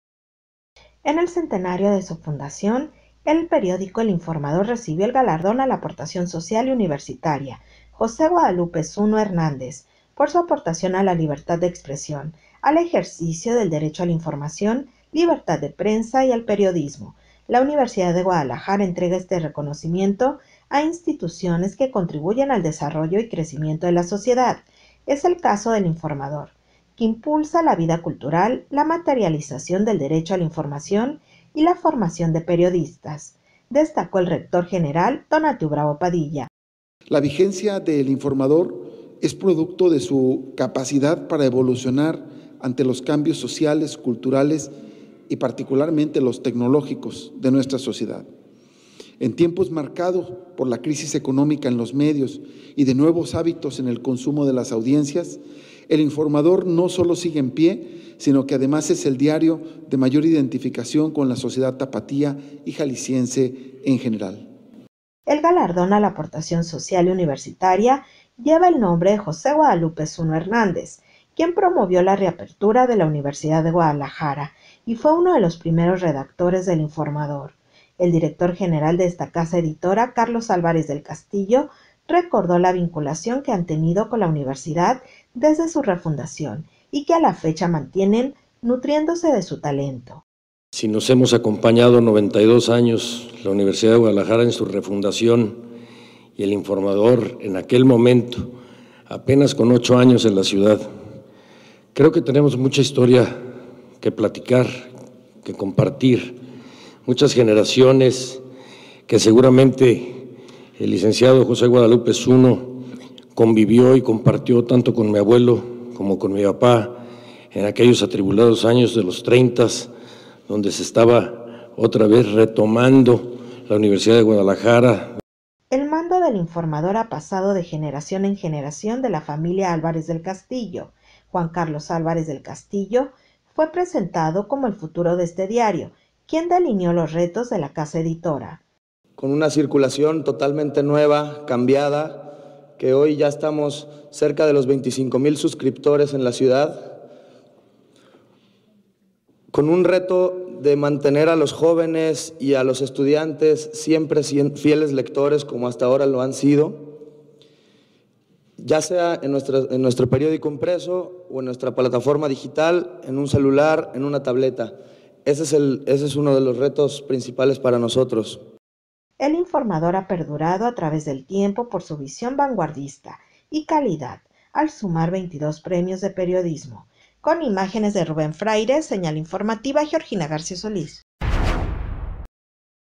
UDG reconoce contribución del periódico El Informador al desarrollo de Jalisco
Universidad de Guadalajara reconoce contribución del periódico El
Informador al desarrollo de Jalisco.